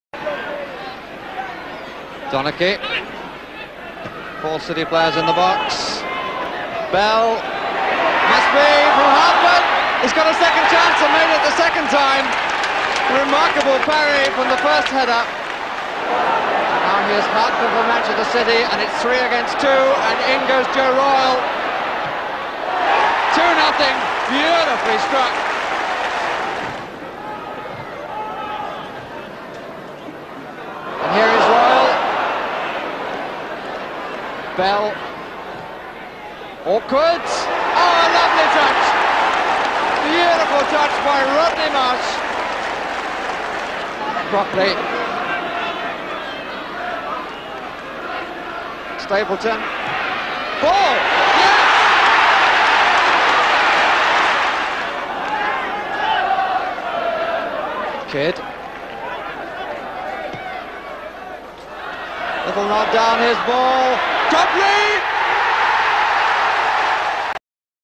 Arsenal Vs Man City, Oct 4th 1975
FLASHBACK TO 1975... Arsenal 2 Manchester City 3
In front of a crowd of 24,928 a Highbury back in the old Division One, Tony Book's City cruised into a 3-0 lead.
Asa Hartford put them ahead, heading in the rebound after his first effort from Colin Bell's cross was parried. Joe Royle scored the second. He took one touch taking the ball just inside into the box to the left of goal before burying it in the top left corner. And Rodney Marsh added a third glancing in another Bell delivery.
But Alan Ball's screamer began Arsenal's fightback. Frank Stapleton sent a pass bobbling over to him and the midfielder struck a first-time shot from 25 yards he struck a first volley off the crossbar and in. Alex Cropley reduced the deficit to just one goal with a clever finish after a flowing move.
If Arsenal had managed to score another it would have extended City's winless run there to 1971 and to 30 matches.
That season City went on to win the League Cup at Wembley, beating Newcastle 2-1 in the final. But Arsenal finished a dismal 17th -- their lowest finish since 1925.